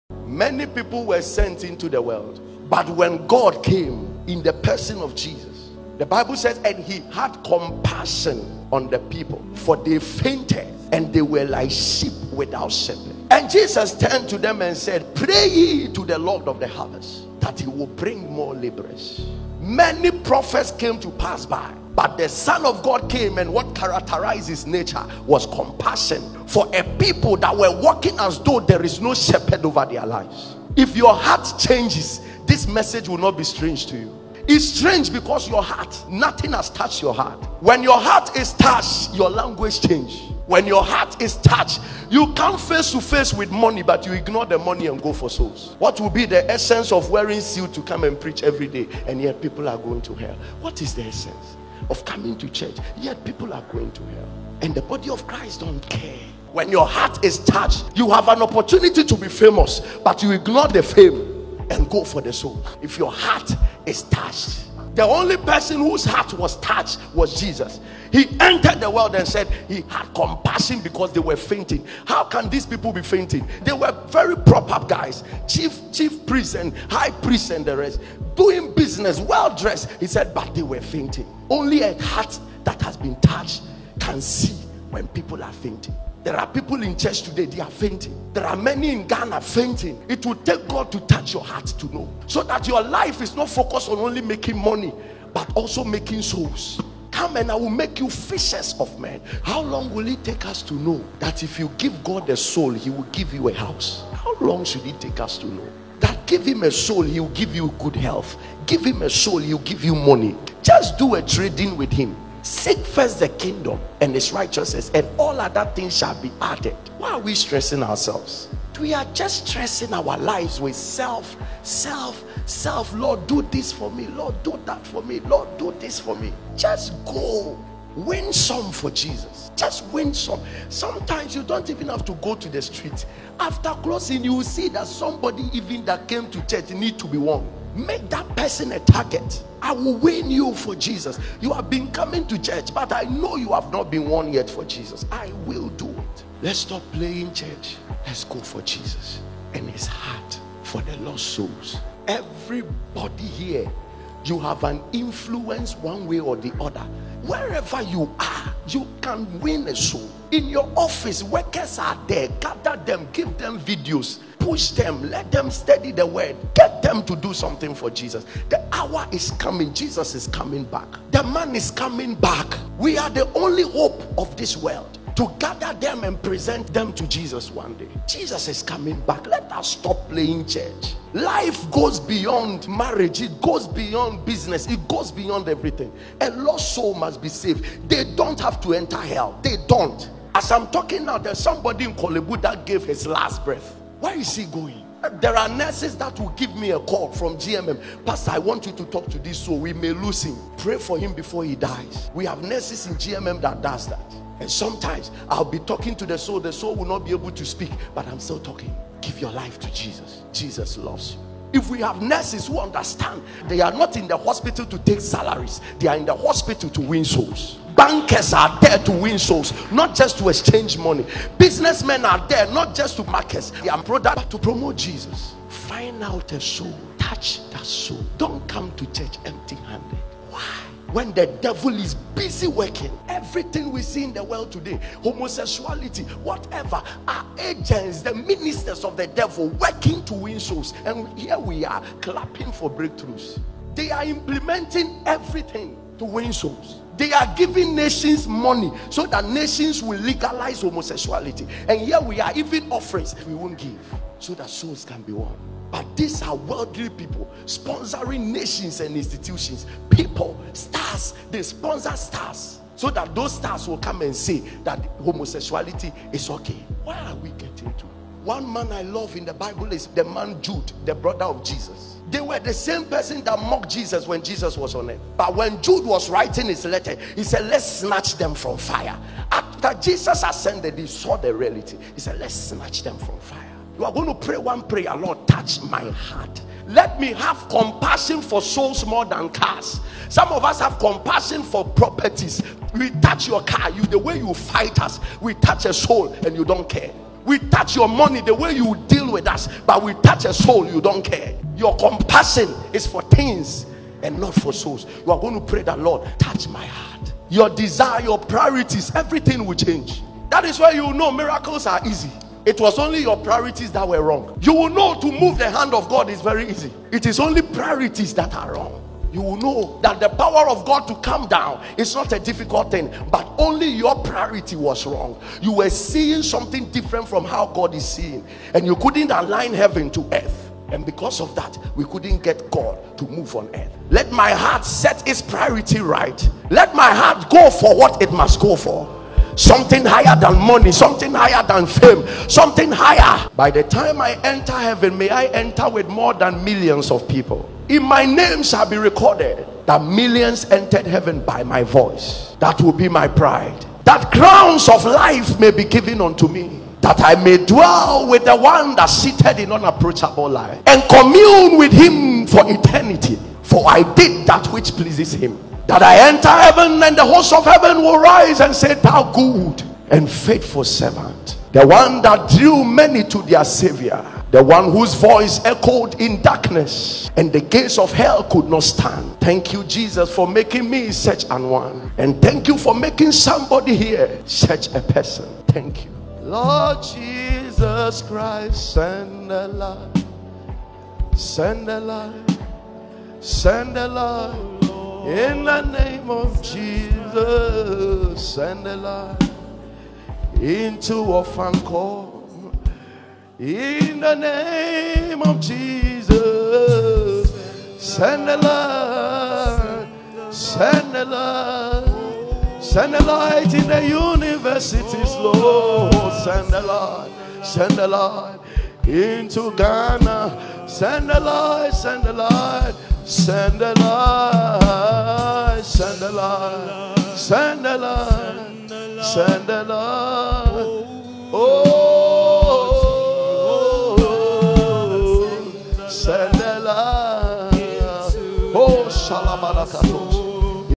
The Secret to Receiving Blessings from God || Pastor Elvis Teaching
We need to have the compassion of Christ. Let stop playing church and go out for souls.